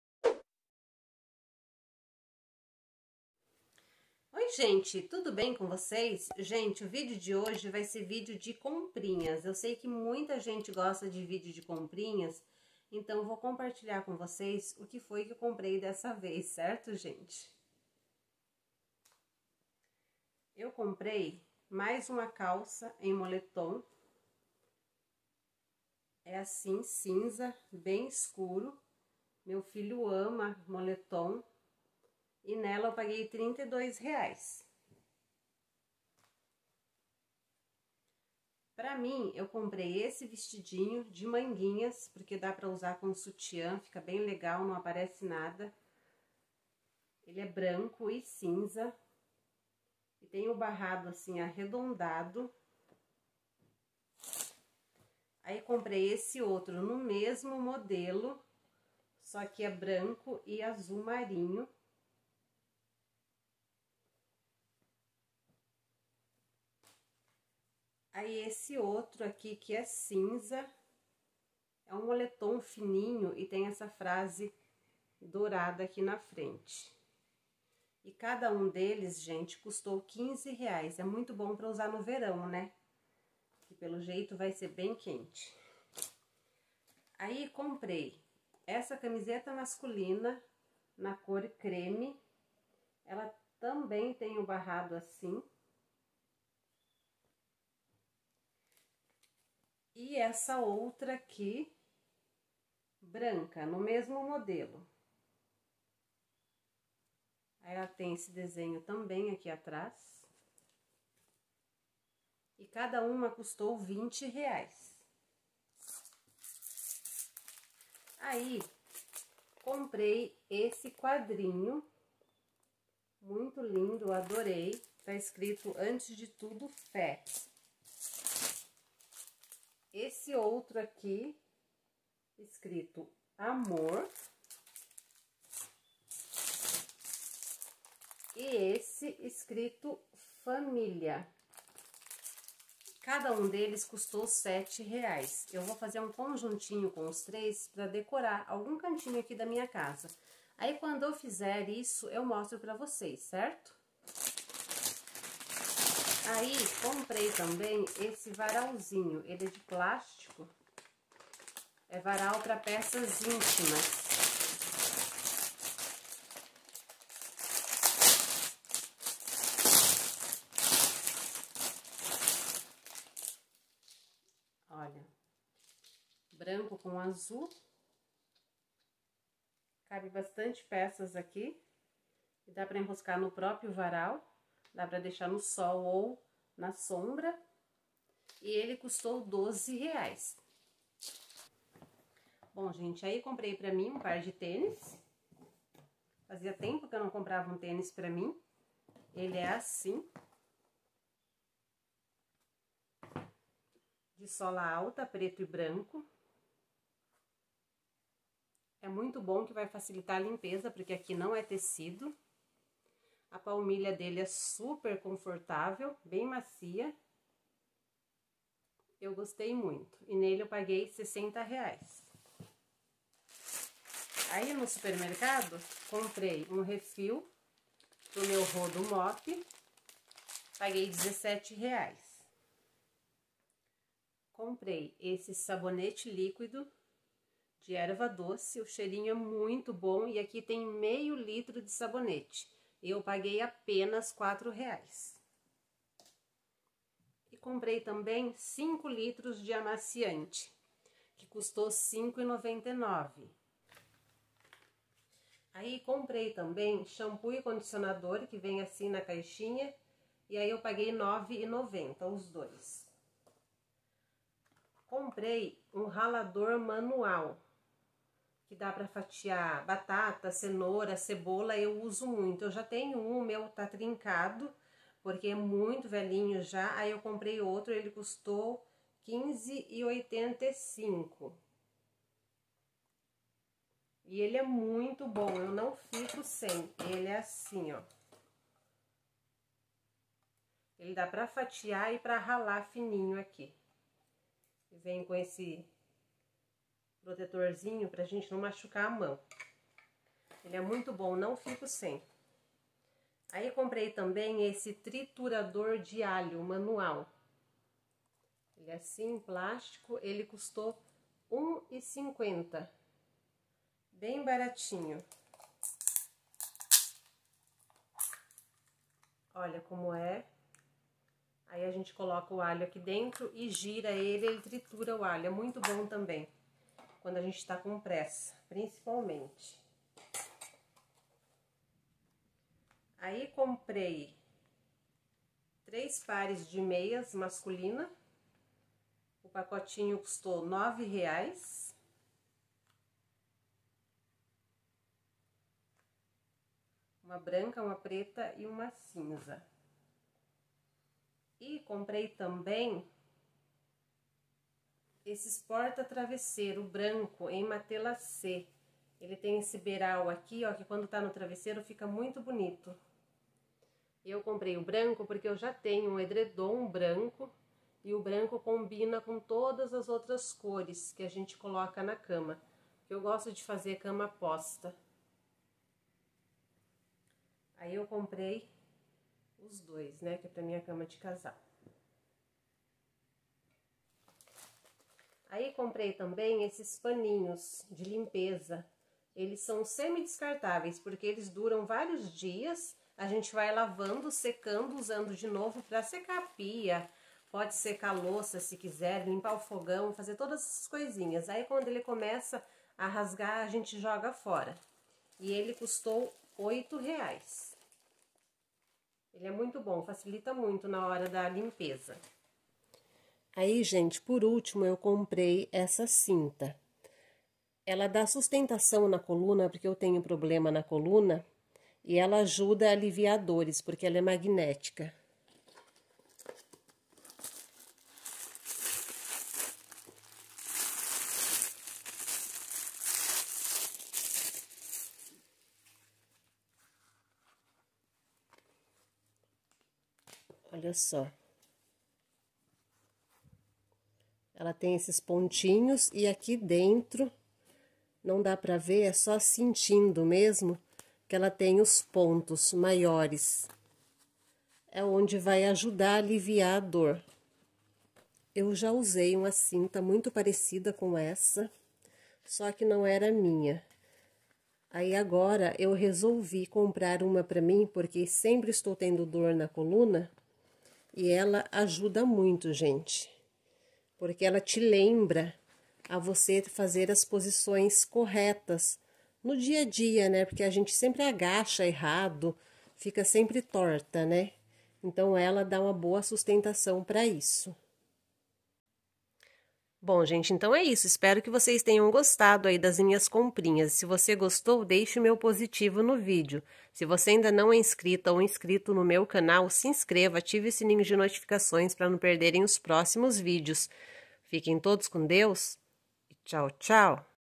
Comprinhas.
Nesse vídeo mostro umas comprinhas q eu fiz. Espero q gostem.

meu Instagram é @silvianunes151
meu Facebook Silvia Nunes.